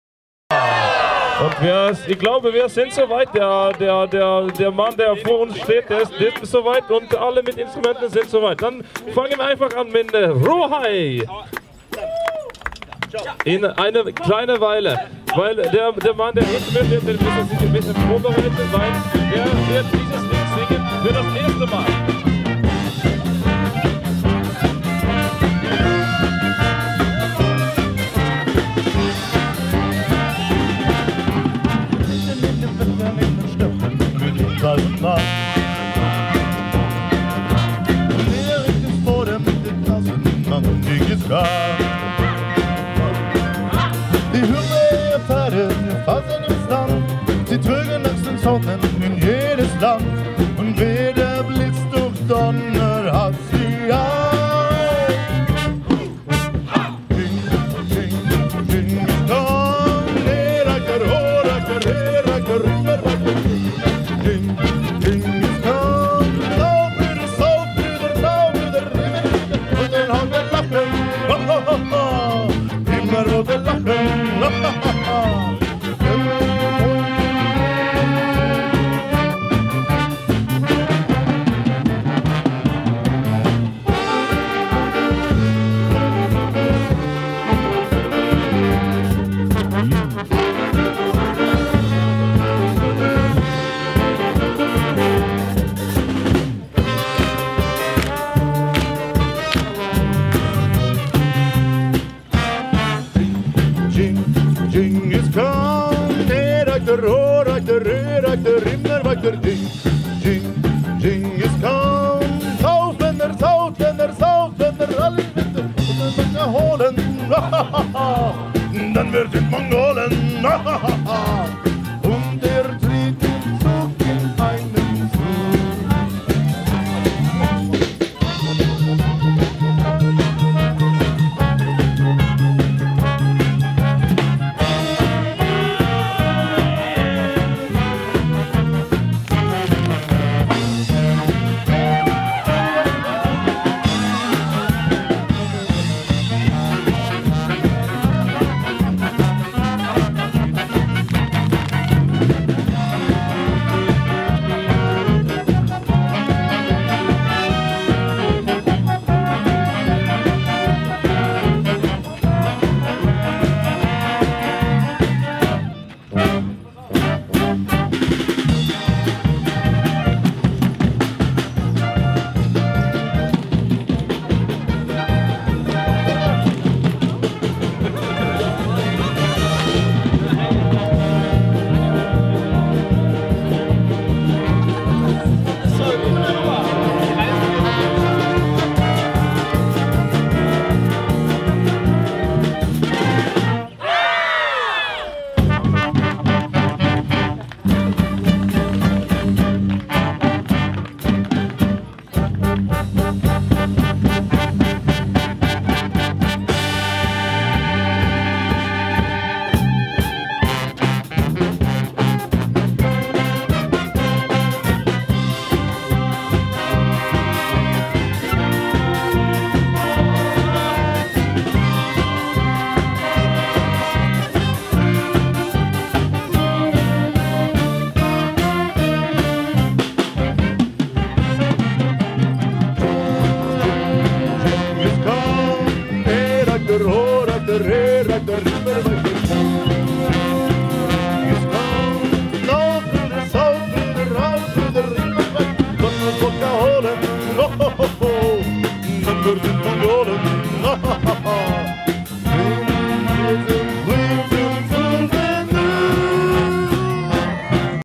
Lid The Blas
Lid The Blas, Sweden
recorded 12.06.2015 at Brückenplatz | Plac Mostowy in Słubfurt, the capital of Nowa Amerika within the project "AKTYWATOR - space for culture | culture for space"